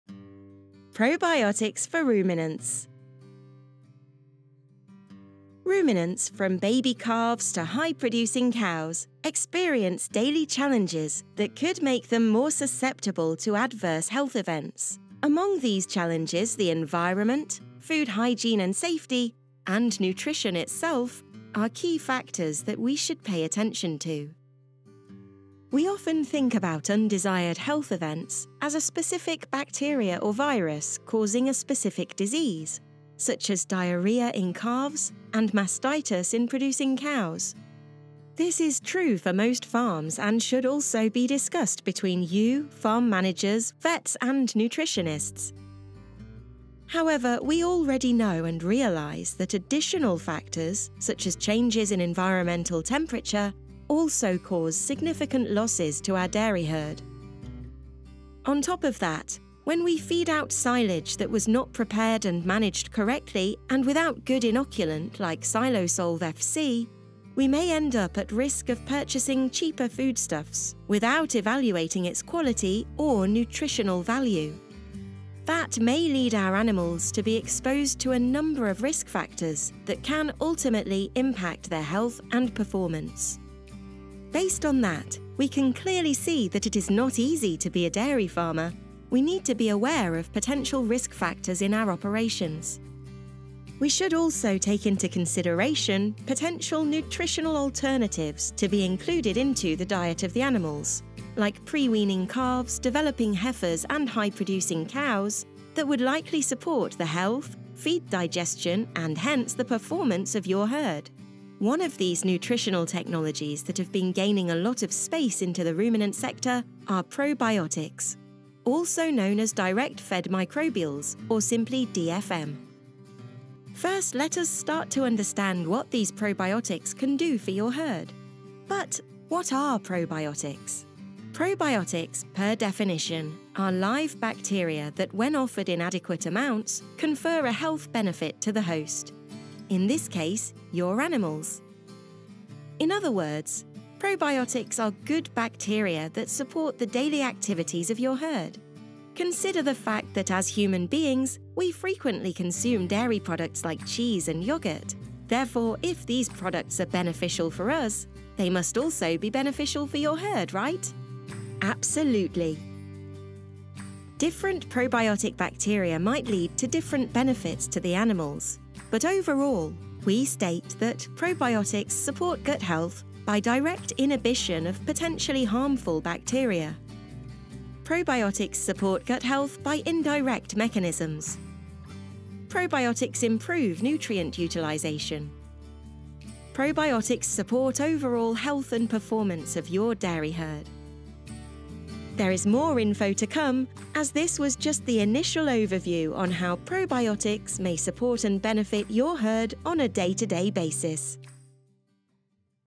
Probiotics for Ruminants
Ruminants, from baby calves to high-producing cows, experience daily challenges that could make them more susceptible to adverse health events.

Among these challenges, the environment, food hygiene and safety, and nutrition itself are key factors that we should pay attention to. Probiotics are best solution to support modern cattle health and performance.